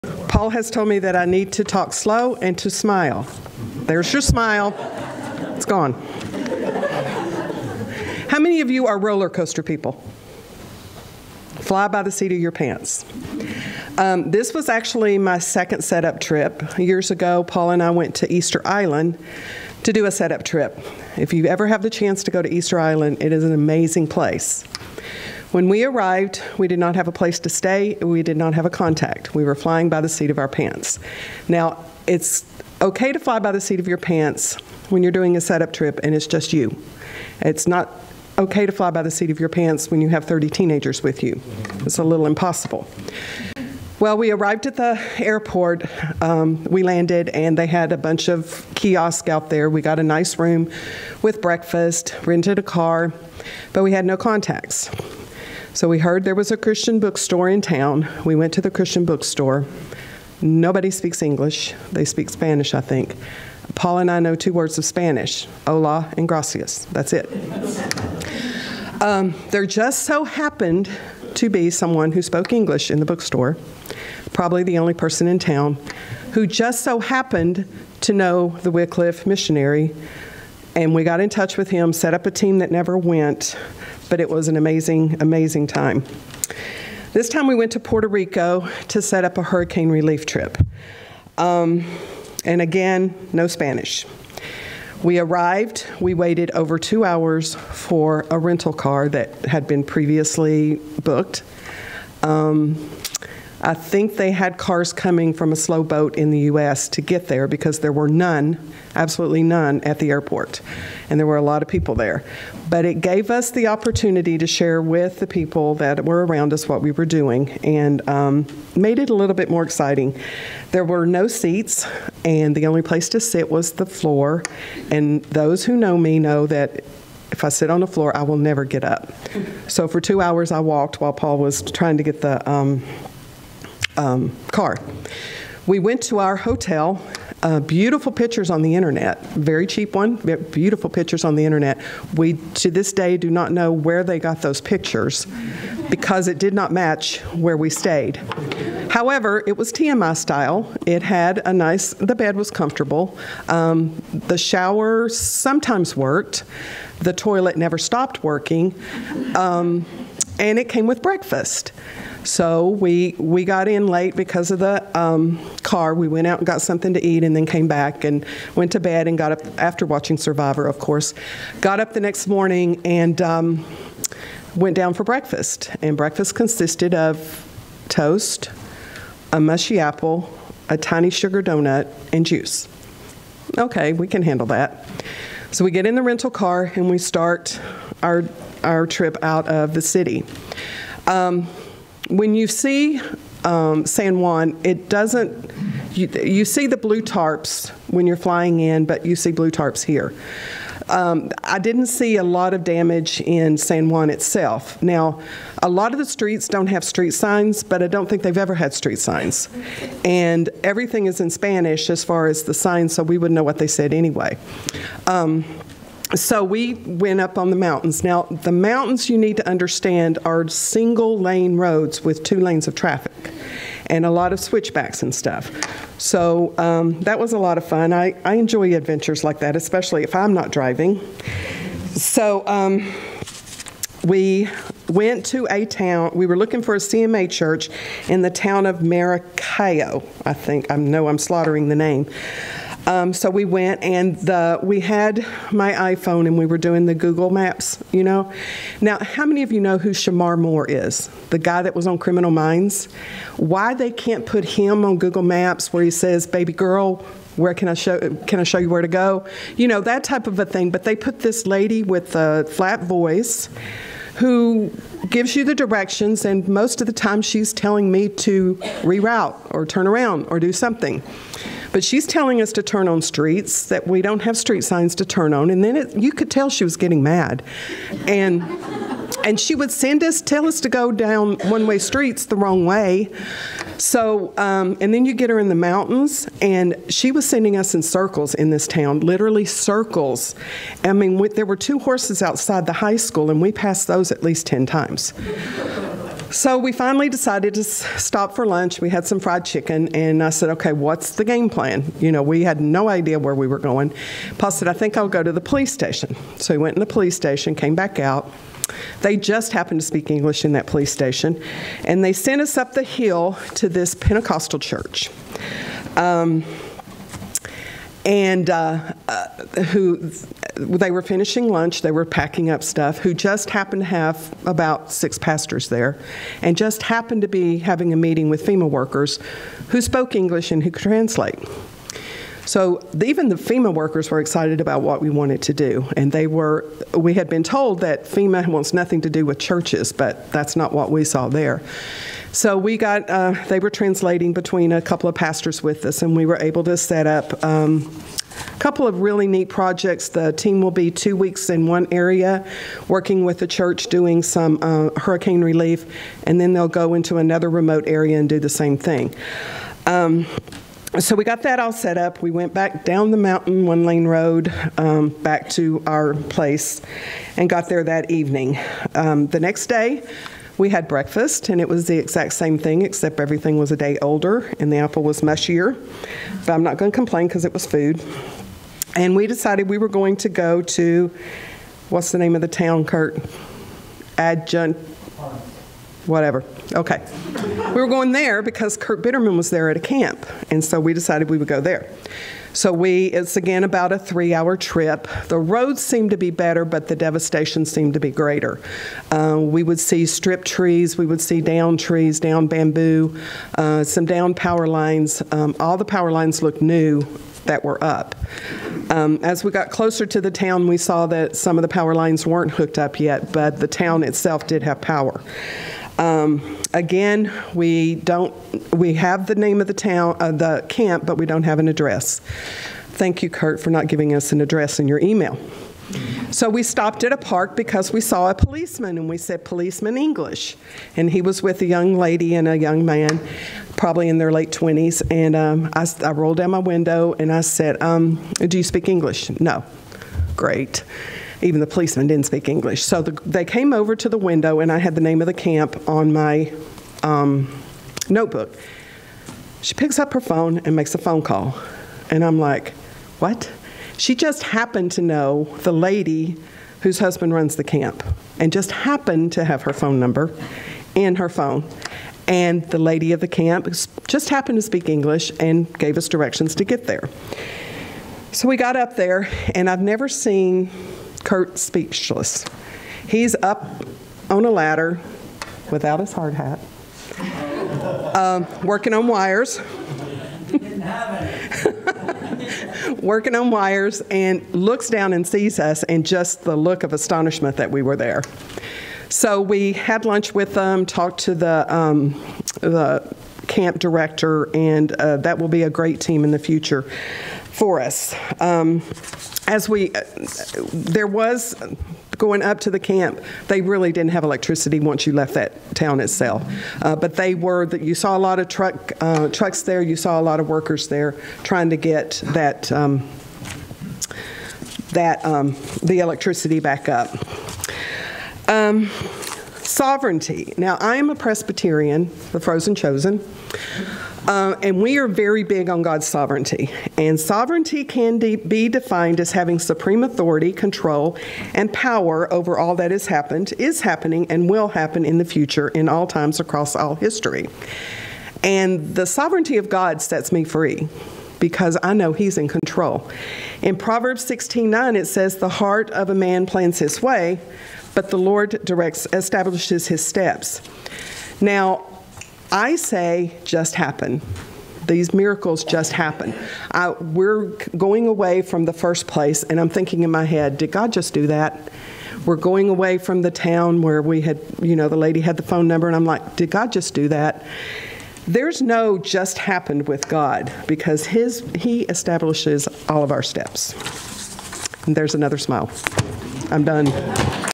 God Moments | Morning Devotions
Was it just a coincidence or was it a 'divine appointment'... a God Moment? Beth Kostner shares some exciting examples from their recent setup trip to Puerto Rico. Enjoy!